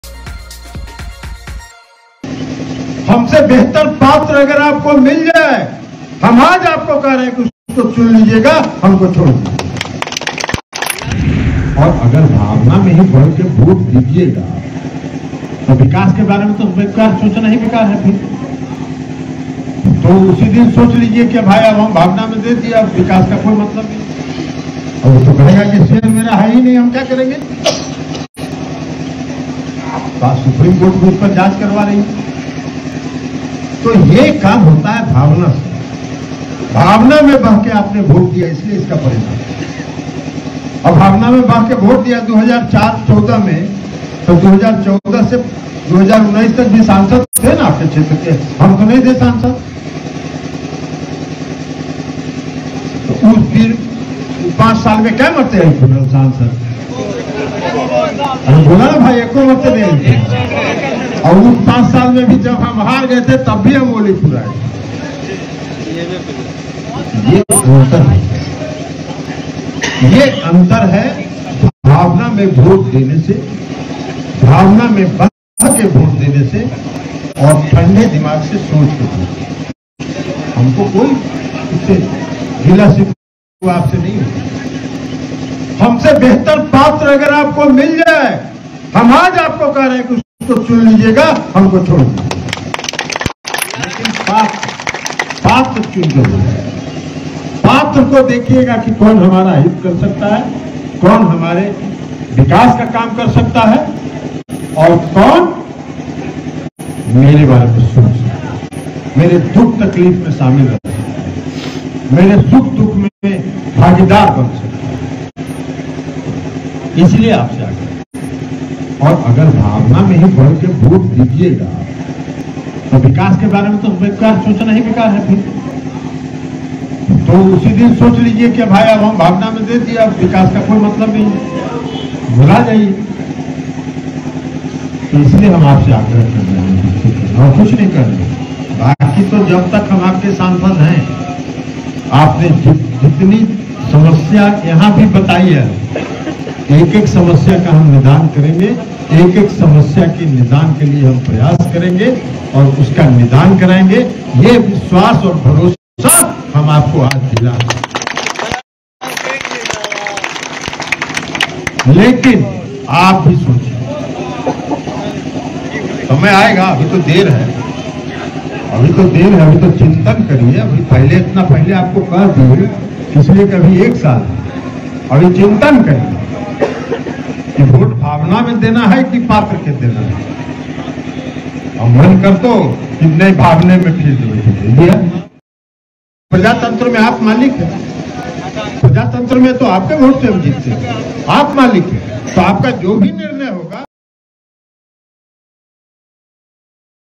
Lalan Singh का SurajBhan Singh की पत्नी व पूर्व सांसद पर करारा हमला, पूछा क्या-क्या किए इलाके में
Lalan Singh का SurajBhan Singh की पत्नी व पूर्व सांसद पर करारा हमला, पूछा क्या-क्या किए इलाके में |News4Nation